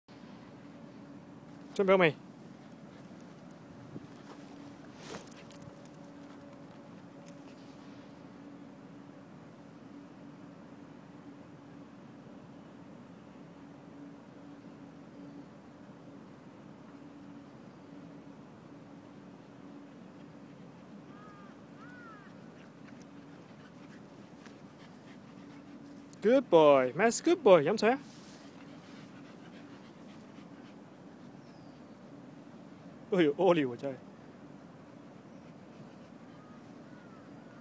Max fetches tennis ball
This was taken on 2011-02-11 at Tokwawan.